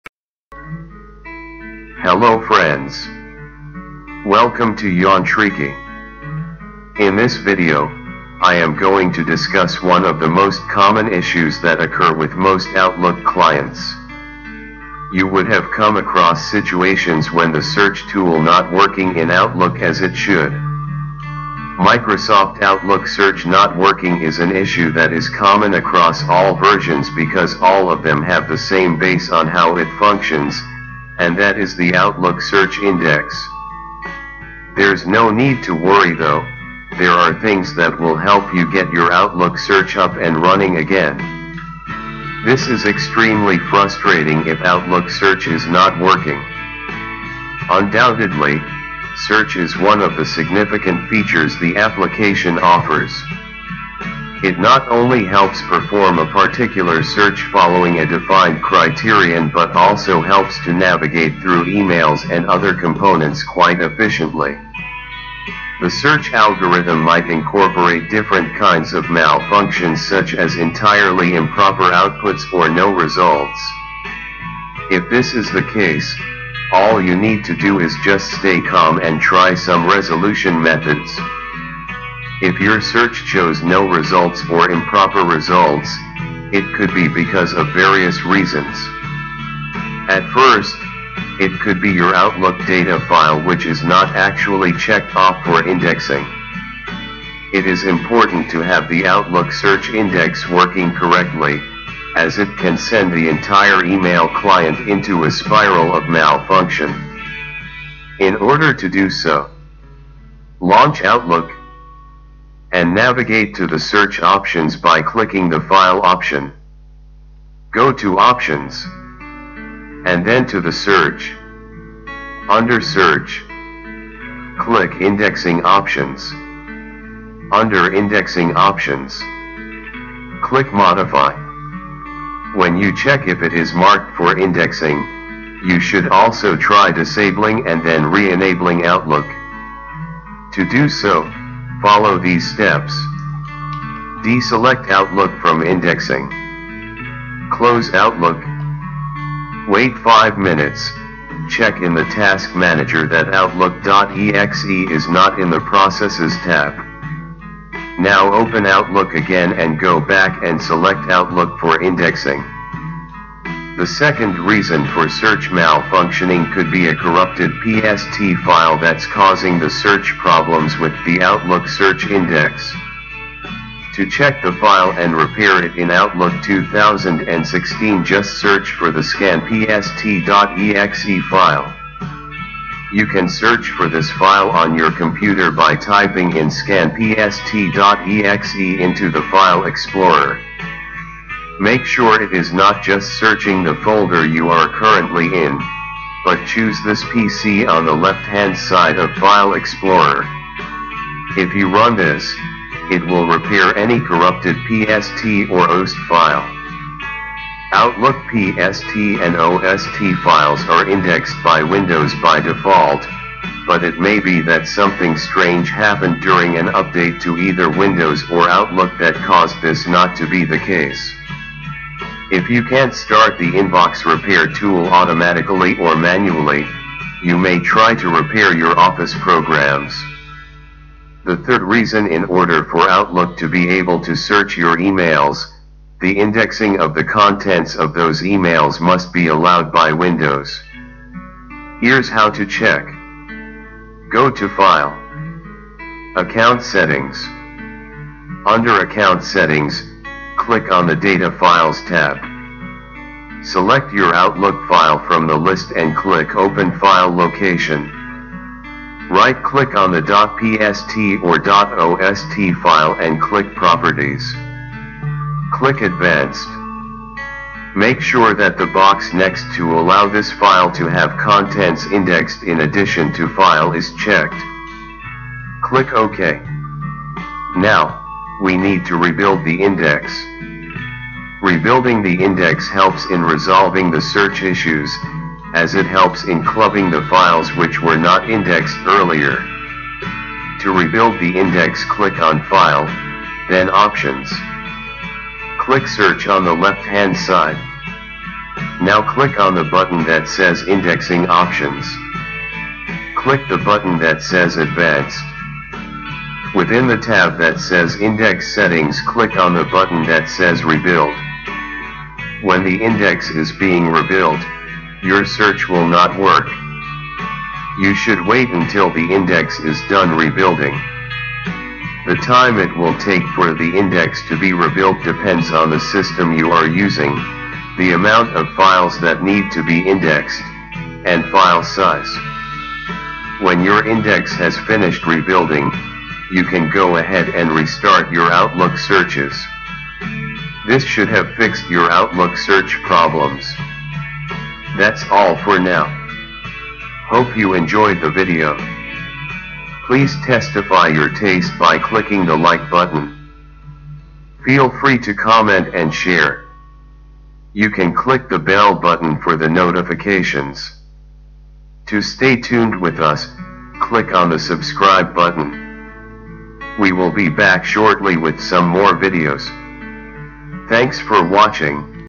Outlook Search Issues
This video is an approach to help you fix the Outlook search issues. This could be helpful for Outlook 2016/2013/2010/2007.